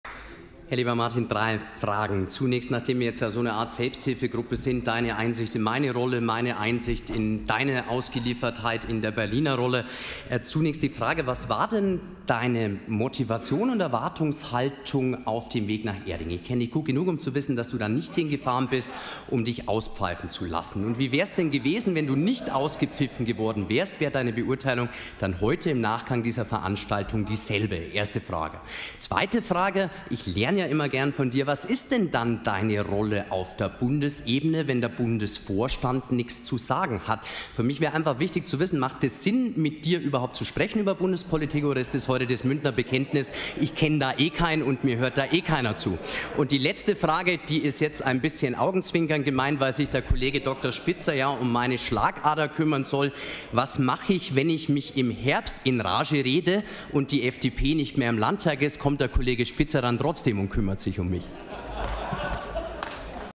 Dr. Fabian Mehring, MdL / 14.06.2023 - Plenum: Zwischenintervention Martin Hagen
Der Parlamentarische Geschäftsführer der FREIEWÄHLER Landtagsfraktion, Dr. Fabian Mehring, MdL, in der Plenarsitzung vom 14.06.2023 im Bayerischen Landtag zur Rede von Martin Hagen - Zwischenintervention zum Dringlichkeitsantrag der BÜNDNIS90/DIE GRÜNEN.